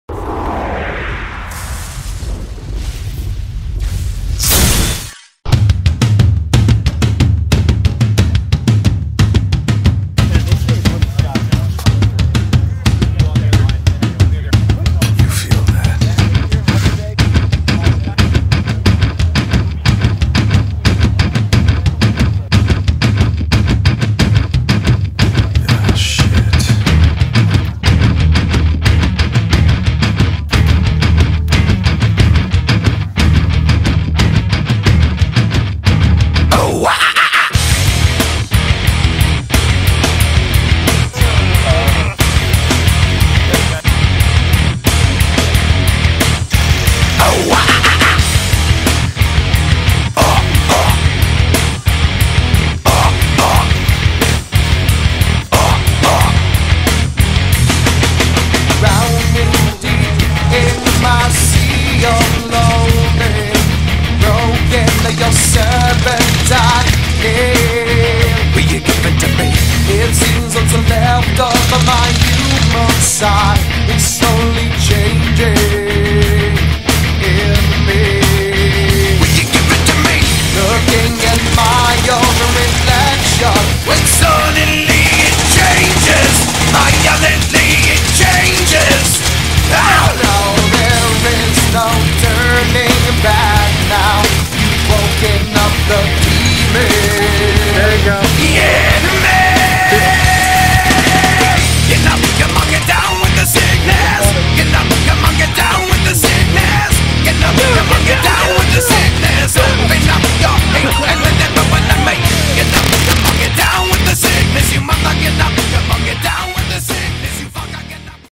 MAA Airsoft Music Video
Many clips from numerous Minnesota Airsoft Association events put together with music.